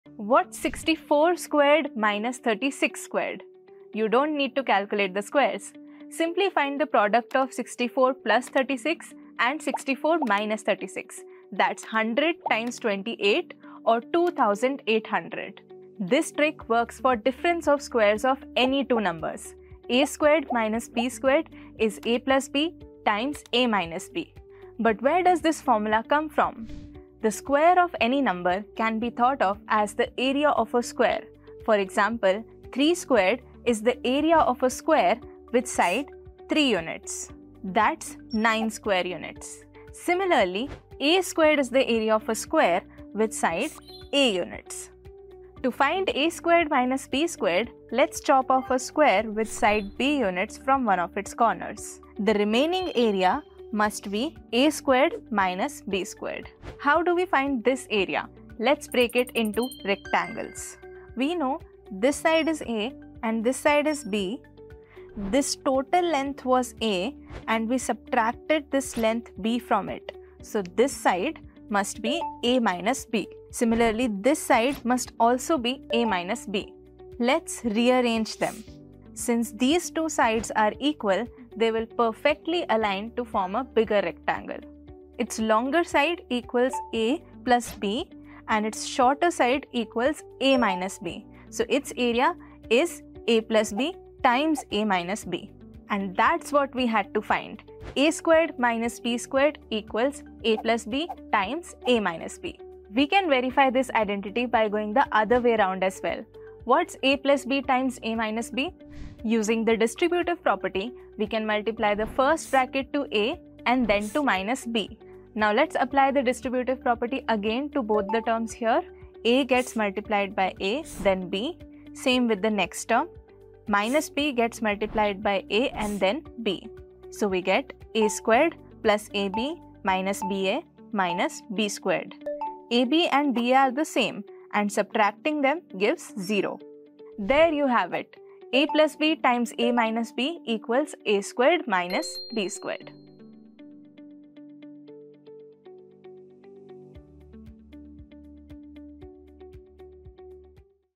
Algebraic Identity a² - b² | Visualization and Proof
We make sense of the algebraic identity a² - b² and derive its expanded form by looking at its geometric interpretation.